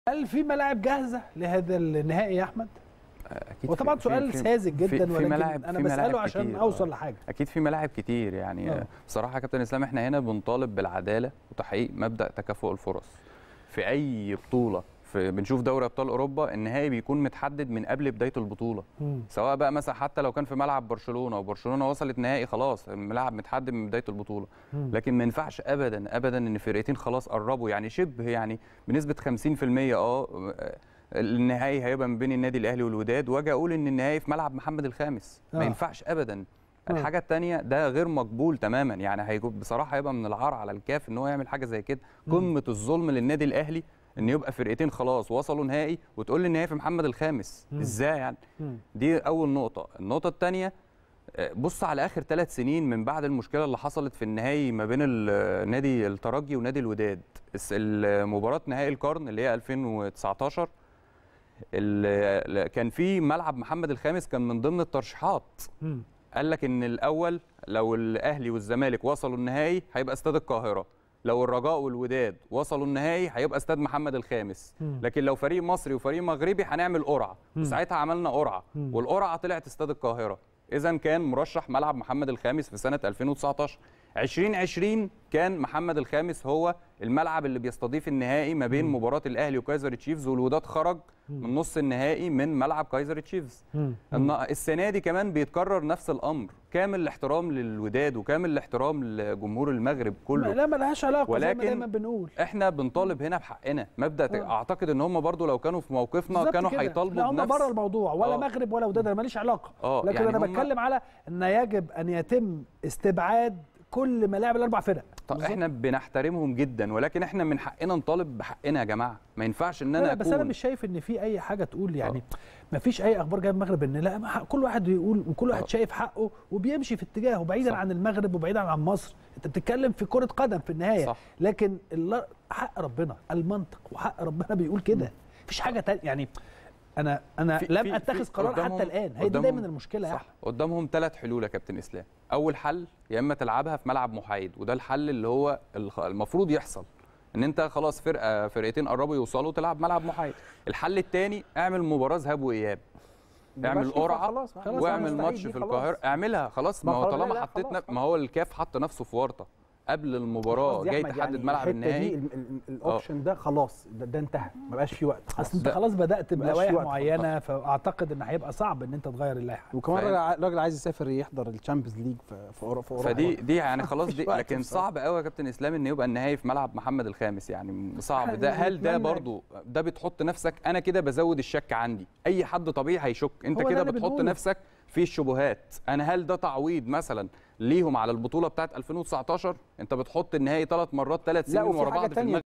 أحمد ثابت: نحن نطالب بالعدلة لتحقيق مبدأ تكافؤ الفرص.. ومن العار على الكاف أن يكون النهائي في المغرب
أحمد ثابت: نحن نطالب بالعدلة لتحقيق مبدأ تكافؤ الفرص.. ومن العار على "الكاف" أن يكون النهائي في المغرب | البيت الكبير